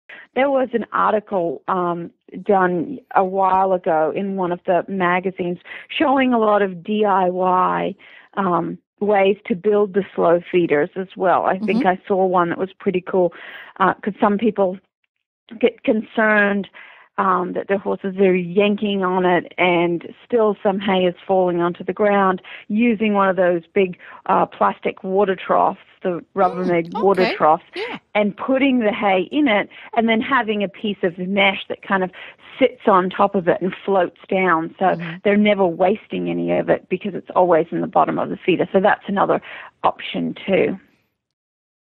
Winter V PHN® on Slow Feeders
Dr. Cubitt of Performance Horse Nutrition continues her discussion with Horse Tip Daily regarding Horse Hay Slow Feeders.  This is the 5th in our 10 part series to prepare for winter.